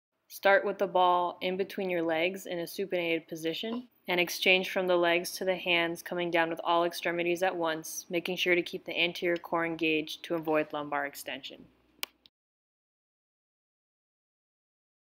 Physioball V Up Exchange
START
- Lying supine on floor with physioball between feet
- Arms outstretched overhead

EXECUTE
- Engage anterior core to lift legs and shoulders off of the ground
- Reach hands to grab ball from legs
- Lower legs and shoulders back to the ground
- Reach ball overhead
- Repeat mechanics on each repetition while passing the ball back and forth between hands and feet

FOCUS
- Use anterior core to move
- Keep head back to maintain a neutral cervical spine

GOAL
- Core strength